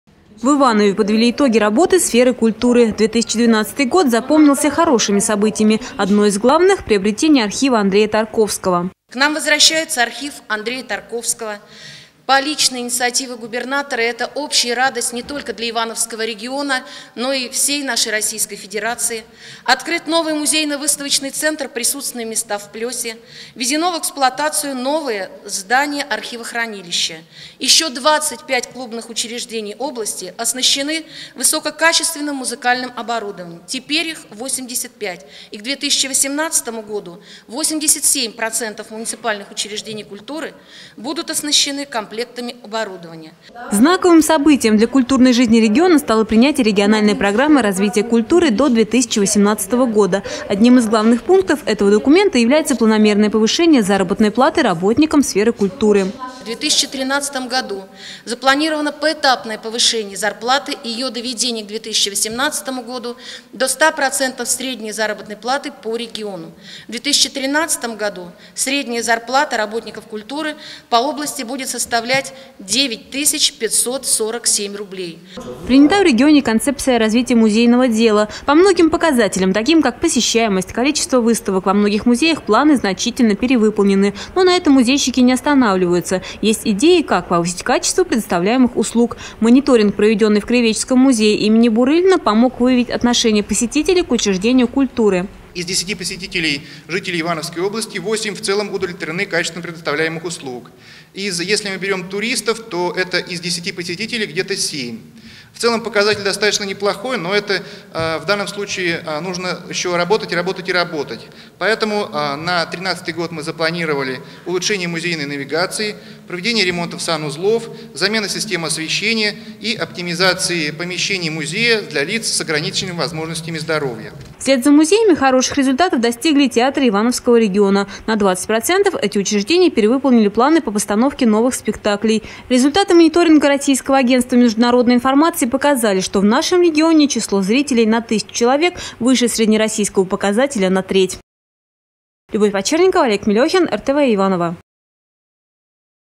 Подведены культурные итоги 2012 года
В Иванове подвели итоги работы сферы культуры в Ивановской области. 2012 год запомнился хорошими событиями. Одно из главных -- приобретение архива Андрея Тарковского.